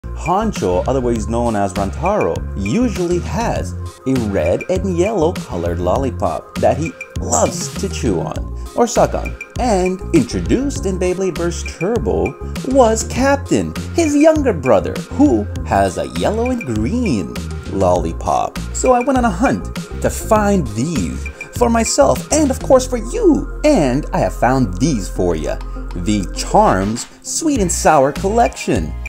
Rantaro and Ranjiro Kiyama LOLLIPOP flavor?
Rantaro and Ranjiro always have a lollipop, I found some that look exactly like them. Complete your cosplay, or feel like the sidekick with your Ragnaruk and a lollipop!(jks) Welcome to the Beyblade dad channel.

for Honchos lollipop

Full episodes of Beyblade Burst Sparking

New Beyblade B-191 Set unbox: "I Burst Every Beyblade in the B-191 set from Takara Tomy "